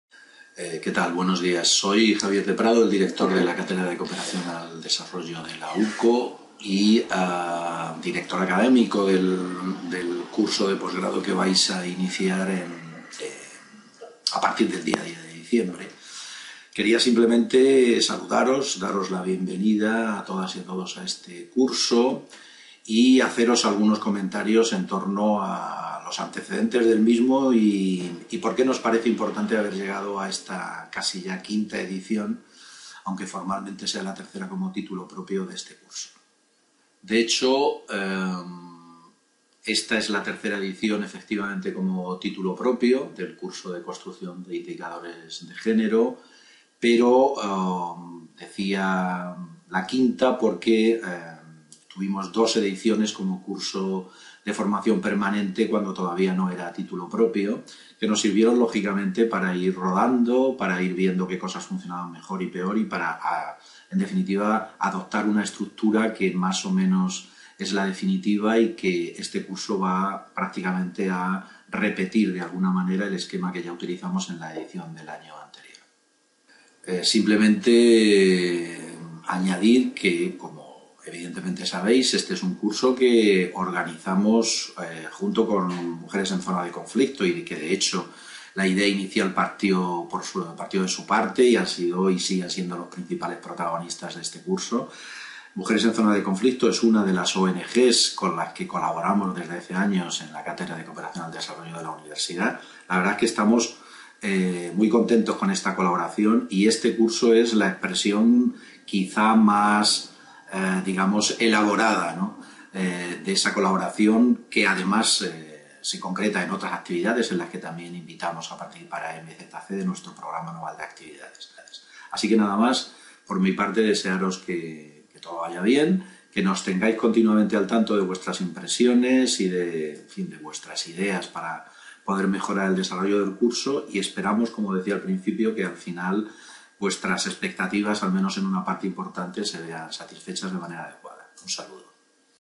Presentacion 3ª Edición Título de Especialista en "Construcción de Indicadores de Género"
Javier de Prado, director de la Cátedra de Cooperación al Desarrollo de la Universidad de Córdoba, da la bienvenida al alumnado del curso de Especialista Universitario "Construcción de Indicadores de Género", que organiza la ONGD "Mujeres en Zona de Conflicto"